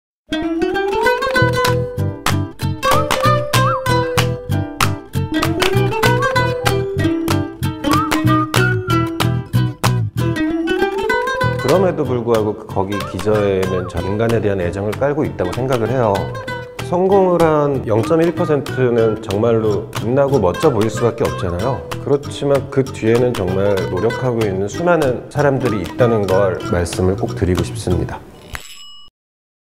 [와콤 X 크리에이터ㅣ이크종] 와콤 모바일스튜디오프로 x 이크종 작가 (short ver.)
'백수 지향 인생'을 모토로 개성 넘치는 그림을 그리는 이크종 작가와 와콤 모바일스튜디오 프로가 만났습니다! 그림을 통해 인간에 대한 애정을 표현하는 이크종 작가의 이야기를 들어볼까요?